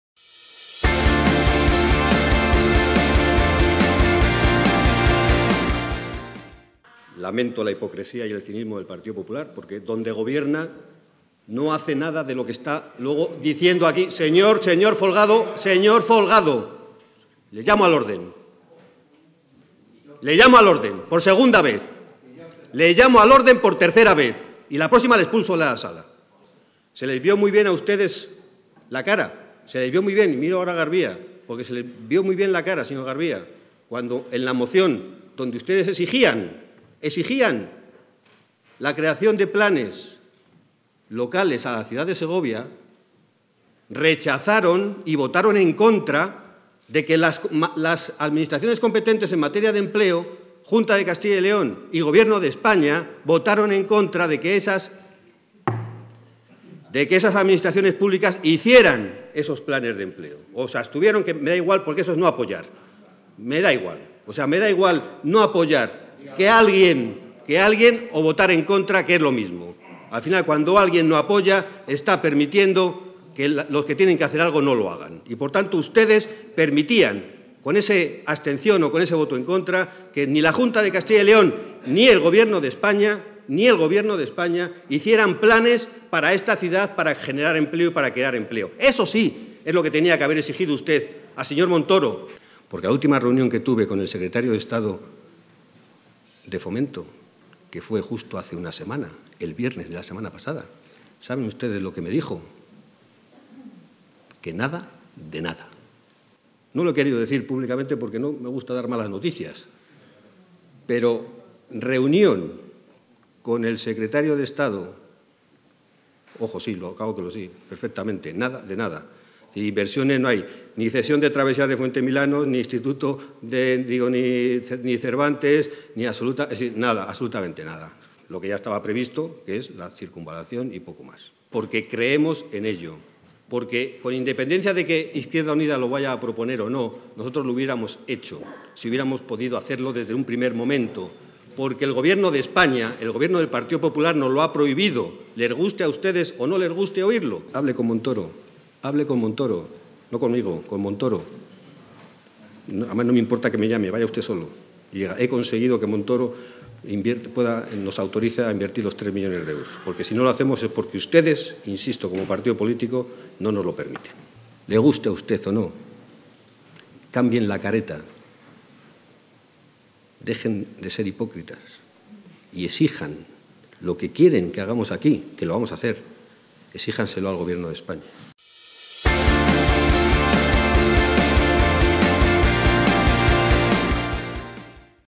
Arahuetes sobre la moción de creación de empleo de IU y en respuesta a PP
Los parados han entrado en el Pleno del Ayuntamiento de Segovia. Los representantes de la Asociación de Parados en Acción de Segovia han soportado las cuatro horas que van desde el inicio, a las 8:15 horas de la mañana del viernes 31 de mayo, hasta la moción de IU que propone destinar el remanente del Presupuesto Municipal, que asciende a unos tres millones de euros, a planes sociolaborales de empleo.